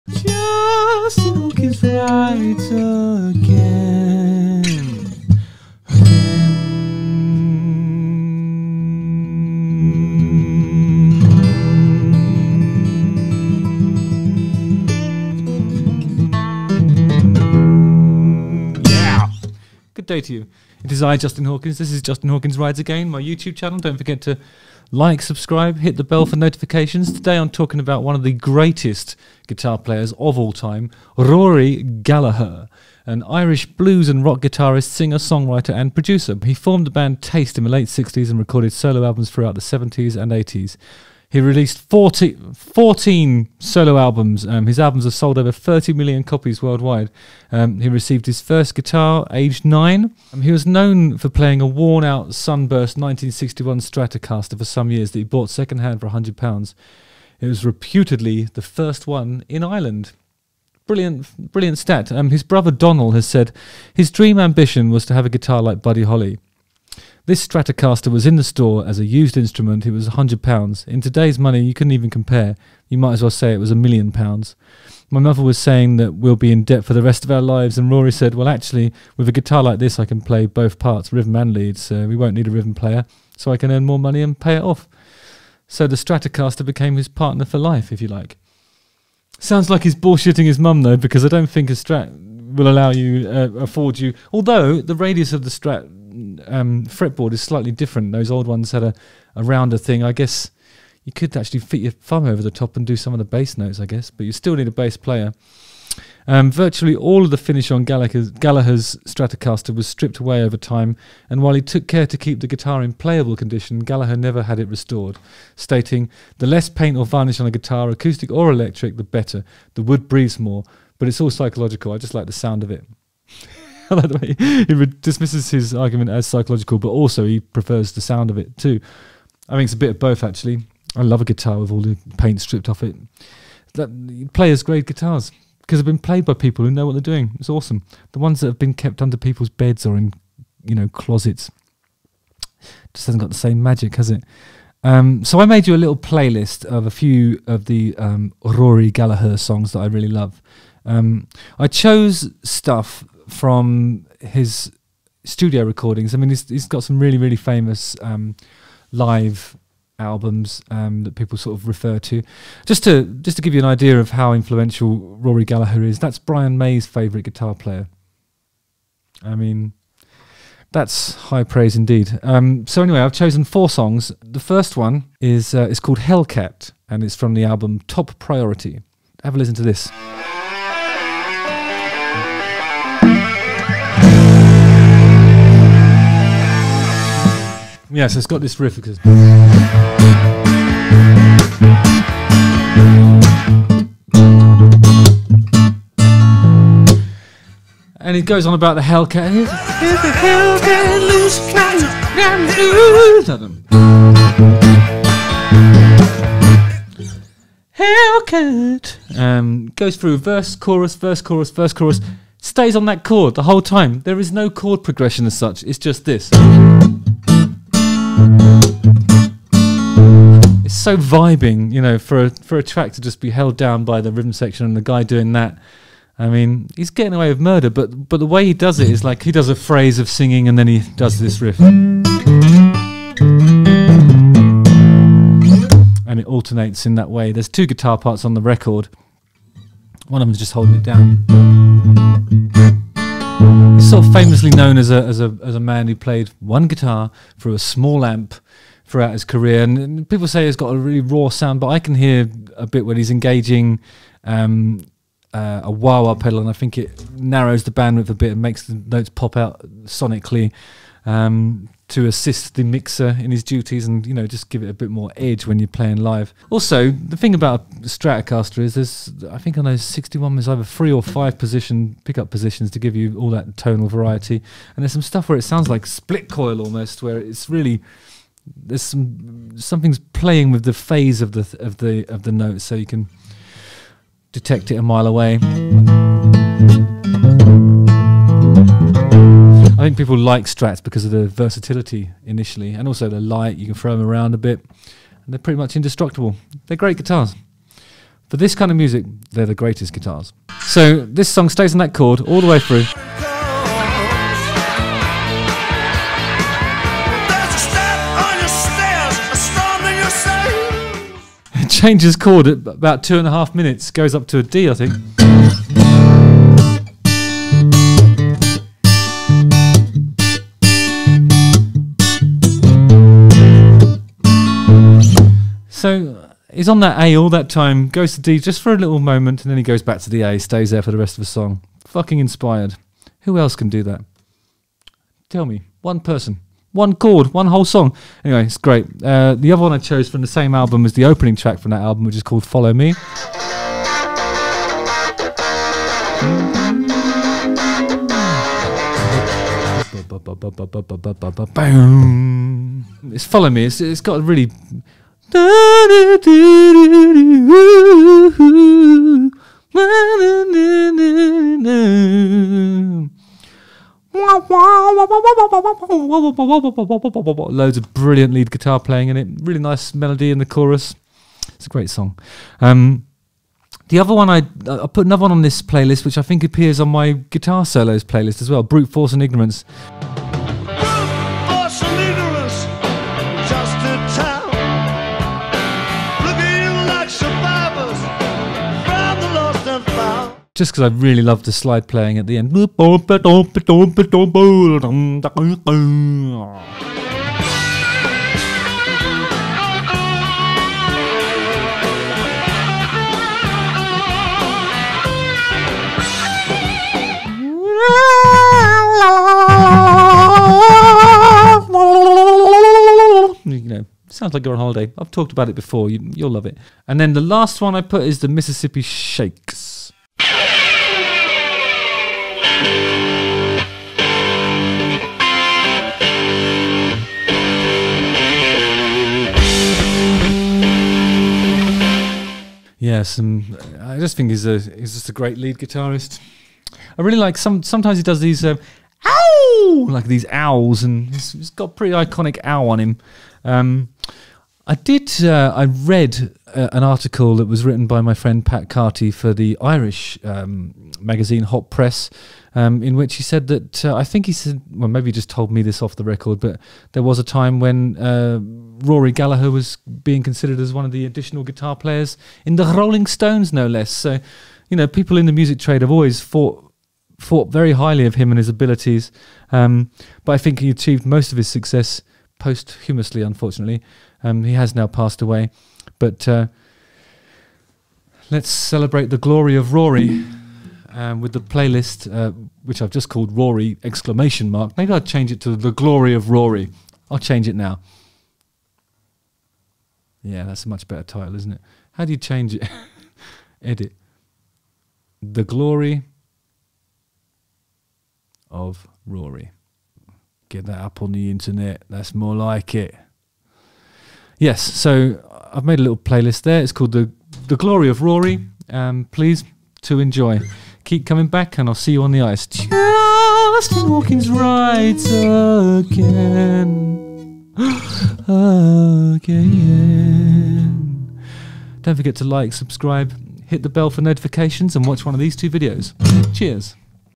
Rory Gallagher Makes Me So Happy.
It's time. It's time for the episode on Rory Gallagher! Rory is such an incredible guitarist. Surprisingly, many aren't aware of him. Please, allow me to share some of my favourite Rory Gallagher tracks so you too can find some happiness through his music.

- Group Zoom Calls
- Live Stream Podcast
- One-on-One Lessons or Chat
- My private demos and music no one else is allowed to hear

I'm on tour in the USA & Canada with The Darkness this year - get tickets